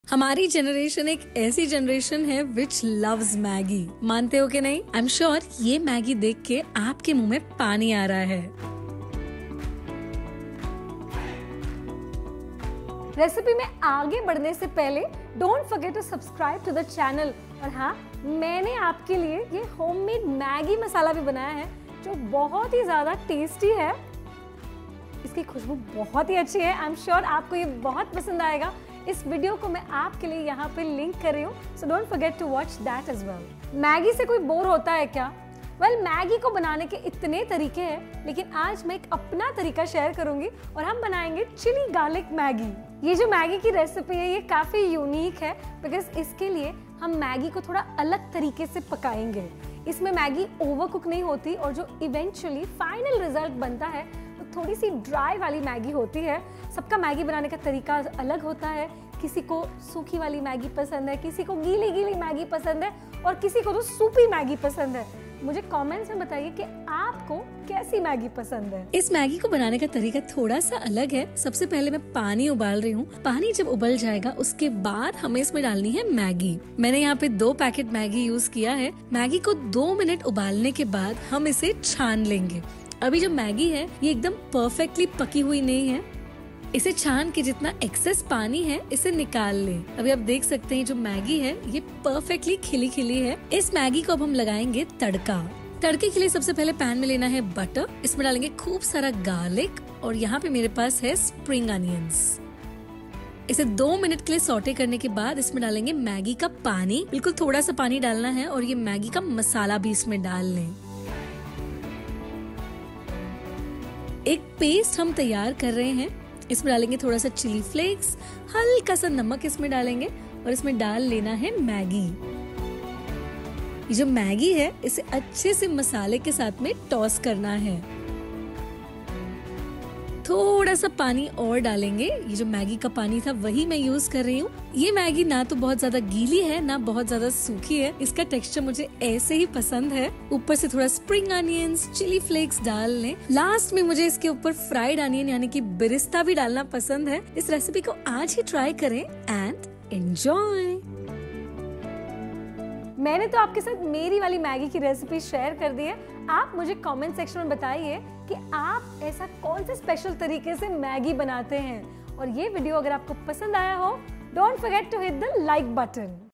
Chilli Garlic Maggi - Quick, Easy & Delicious Recipe | चिल्ली गार्लिक मॅग्गी आसान रेसिपी
Today we’re going to make the regular Maggi with a twist. It’s the most delicious & flavourful Maggi packed with garlic & spices! If you’re a lover of this combination, this video is for you & you got to try it! ❤️

• 1 pack Maggi noodles
• 1 packet Maggi masala
• 2 tbsp Chopped garlic cloves
• 1 tsp chilli flakes
• 2 tsp Oil
• Chopped spring onion greens
• Fried onions/ birista, for garnish

Method-
1. To start with, boil the maggi noodles, strain and set aside. Also, reserve the strained water.
2. Now, heat oil in a pan. Add in garlic, maggi masala, spring onions and saute well. Add in the noodles and mix.
3. Season with salt, add chilli flakes, some reserved water and toss well.
4. Remove, garnish with spring onions, chilli flake, birista and serve hot.
5. Enjoy!